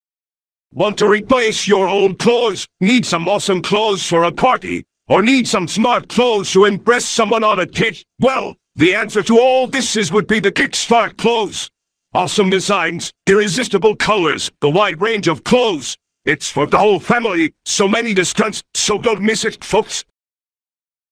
The KickStart Clothes (my ad concept!)
Meiko55 has wrote the script for the KickStart clothes ad, so I decided to make a concept for it! It's very kicking, isn't it? The clothes ad may be a very kicking viral meme.

Because I don't have a desktop microphone yet, then I used Vocodes to make Yami Yugi (Dan Green) say the KickStart clothes ad script.

Software(s) used: Vegas Pro 18, Photoshop and Vocodes

Super Robot Monkey Team Hyperforce Go! is the property of Disney​​ and Ciro Nieli. Credit to Meiko55 for the KickStart clothes ad script. The ad logo is made by me.